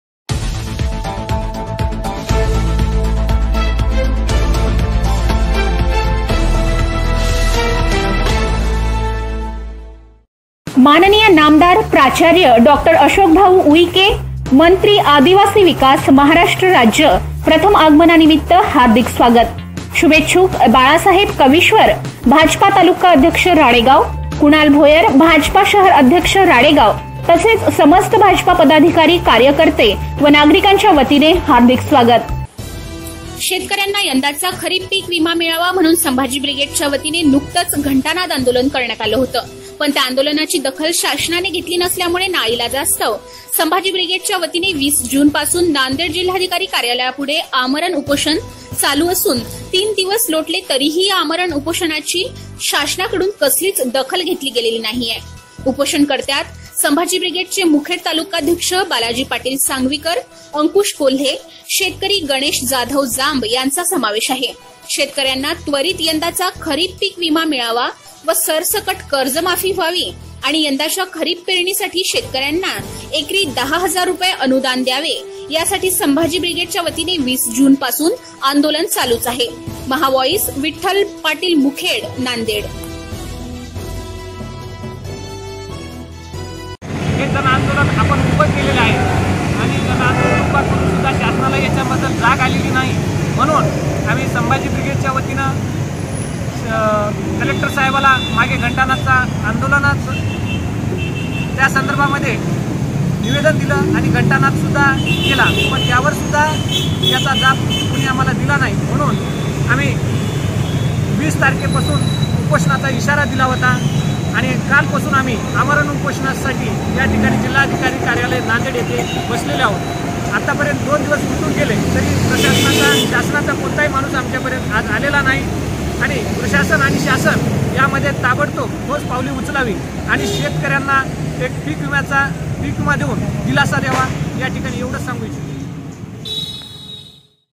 नांदेड जिल्हाधिकारी कार्यालयापुढे संभाजी ब्रिगेडचे आमरण उपोषण चालूच...
मुखेड - नांदेड ( मराठवाडा )
विठ्ठल पाटील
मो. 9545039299

शेतकऱ्यांना यंदाचा खरीप पिक विमा मिळावे म्हणून संभाजी ब्रिगेडच्या वतीने नुकतच घंटानाद आंदोलन करण्यात आले होते. पण त्या आंदोलनाची  दखल शासनाने घेतली नसल्यामुळे नाईलाजास्तव संभाजी ब्रिगेडच्या वतीने  20 जून पासून नांदेड जिल्हाधिकारी कार्यालयापुढे आमरण उपोषण चालू असून तीन दिवस लोटले तरी याही आमरण उपोषणाची   शासनाकडून कसलीच दखल घेतली गेली नाही. उपोषण कर्त्यात संभाजी ब्रिगेडचे मुखेड तालुकाध्यक्ष बालाजी पाटील सांगवीकर, अंकुश कोल्हे , शेतकरी गणेश जाधव जांब , यांचा समावेश आहे . शेतकऱ्यांना त्वरित यंदाचा खरीप पिक विमा मिळावे व सरसकट कर्जमाफी व्हावी आणि यंदाच्या खरीप पेरणीसाठी शेतकऱ्यांना एकरी दहा हजार रुपये अनुदान द्यावे यासाठी संभाजी ब्रिगेडच्या वतीने 20 जून पासून.  आंदोलन   चालूच   आहे.   महा व्हाॅईस  ‌न्यू‌‌जसाठी  विठ्ठल‌  पाटील  मुखेड - नांदेड